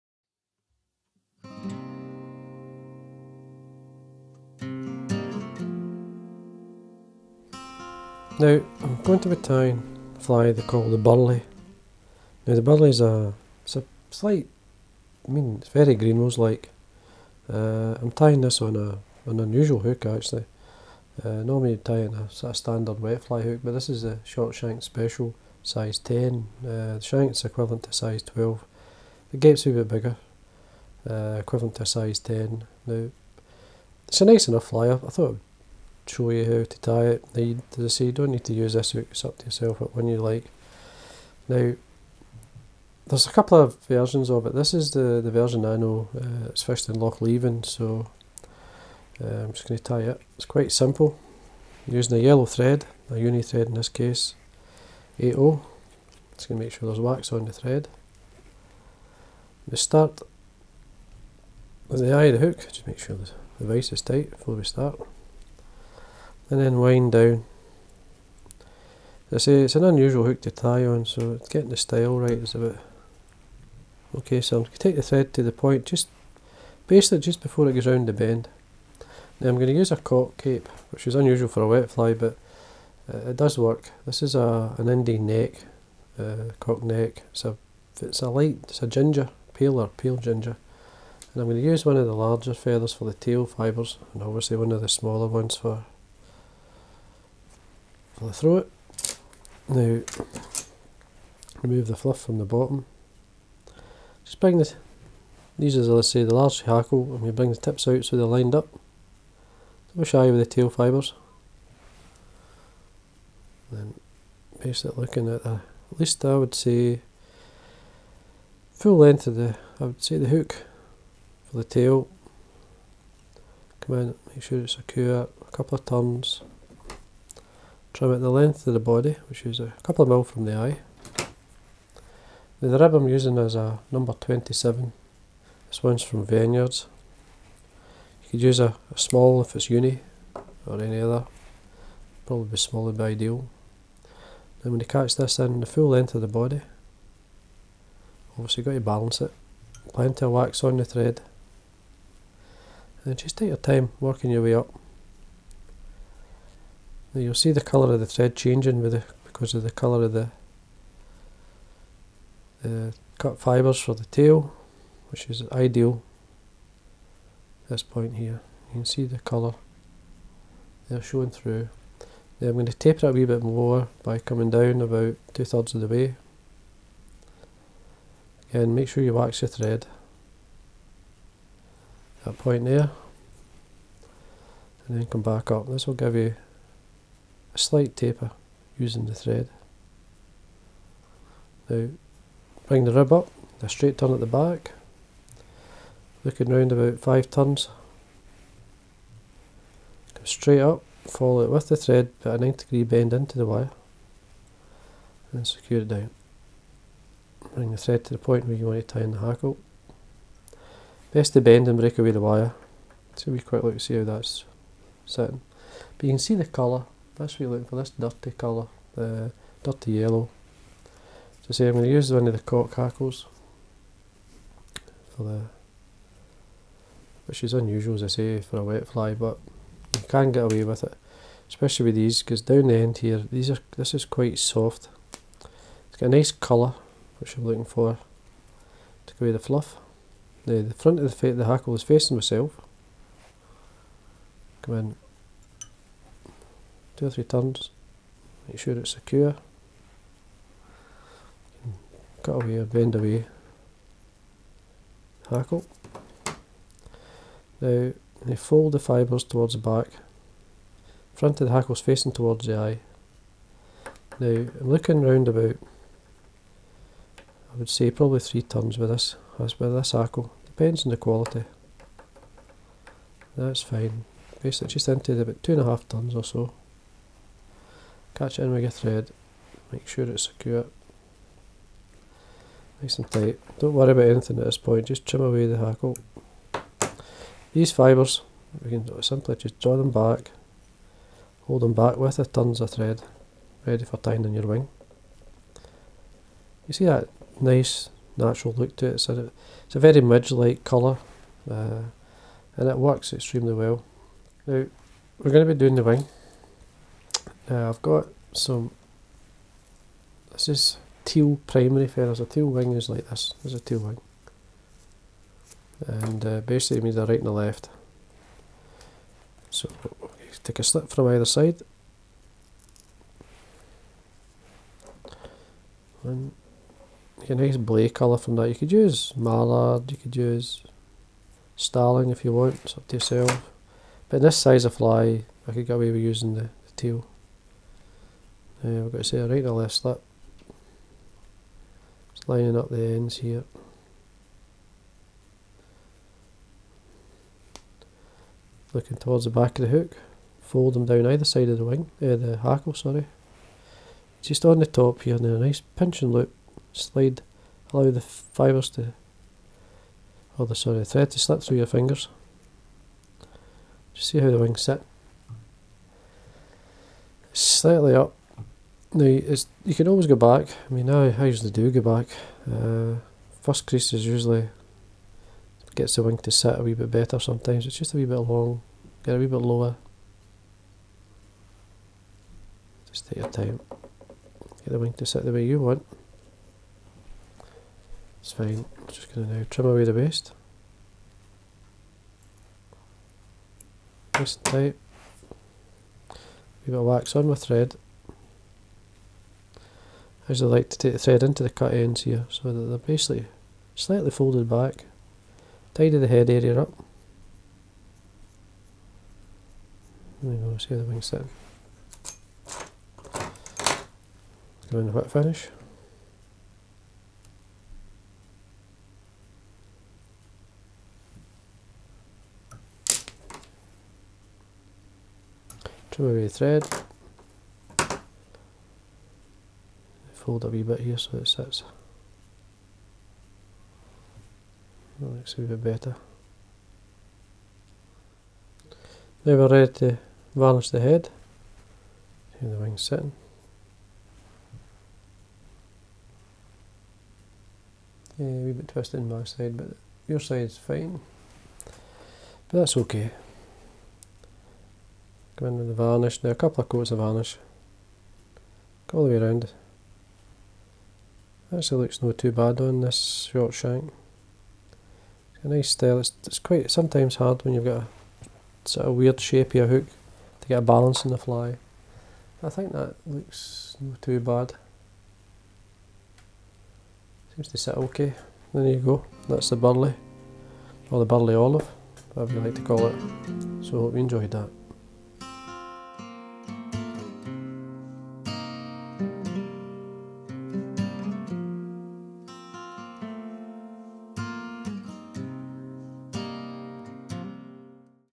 Tying the Burleigh Olive with Davie McPhail
Materials Used;

Hook, Fulling Mill Short Shank Special size 10
Thread, Uni-8/0 Yellow
Tail, Light Ginger Hen or Cock
Rib, Veniards No.27 Silver Wire or Small
Body, Yellow Thread
Hackle ,Light Ginger Hen or Cock
Wing, Teal Primary